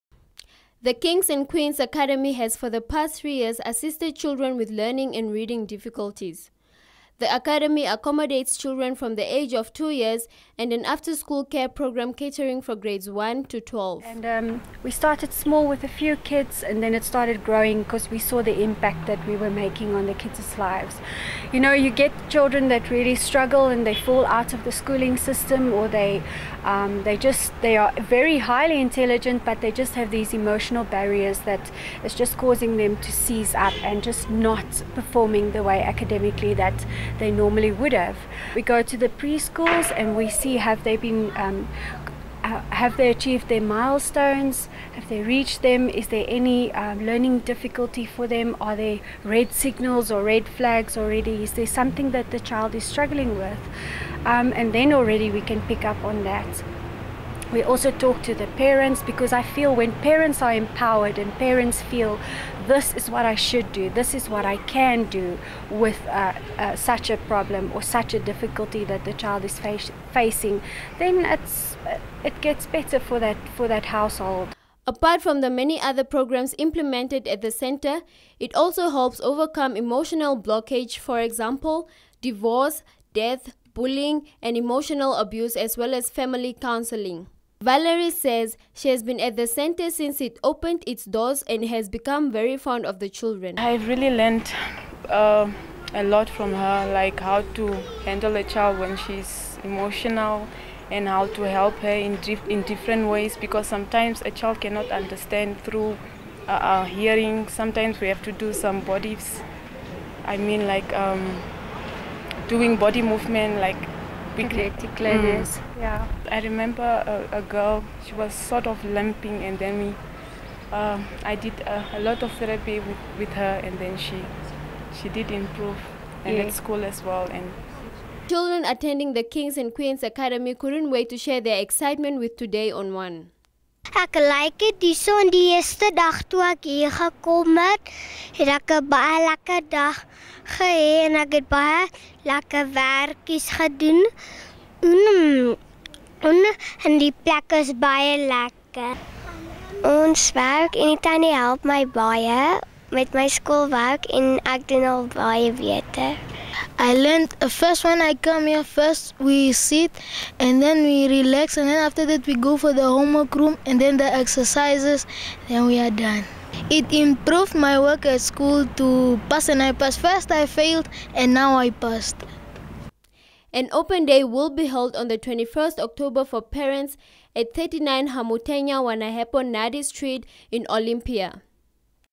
Kings and Queens reaches out to children with special needs. By: Selma Taapopi
Celebrating three years of amazing achievements, the Kings and Queens Academy since its inception, has changed the lives of many children and families. The KQ Academy has for the past three years assisted children with learning and reading difficulties. The Academy accommodates children from the age of 2 years and an after-school care program catering for Grades 1 to 12. Apart from the many other programs implemented at the centre it also helps overcome emotional blockages for example divorce, death, bullying and emotional abuse as well as family counseling. Valerie said she has been at the centre since it opened its doors and has become very fond of the children. Children attending the KQ Academy couldn’t wait to share their excitement with Today on One. An open day will be held on 21 October for parents at 39 Hamutenya Wanahepo Ndadi Street in Olympia.